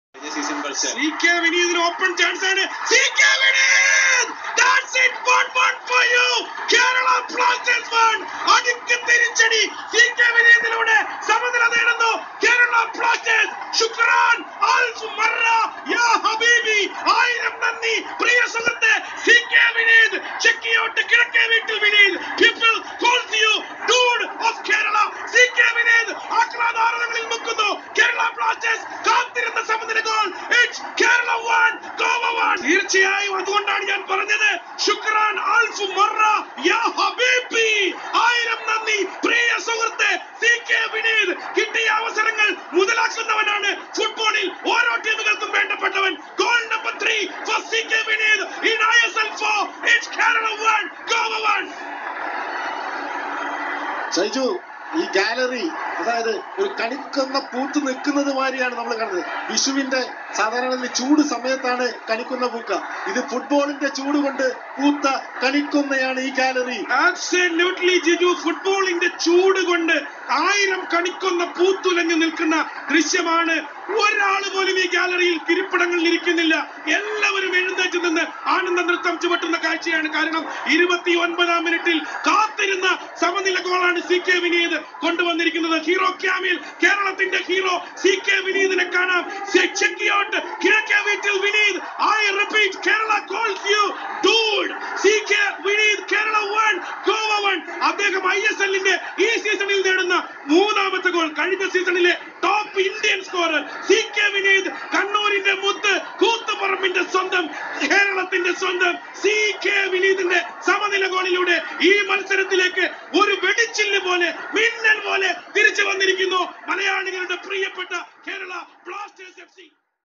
CK Vineeth Goal Hero ISL | Shaiju Damodaran Mass Commentary
Malayalam Best commentary Of Shaiju Samodaran ISL kerala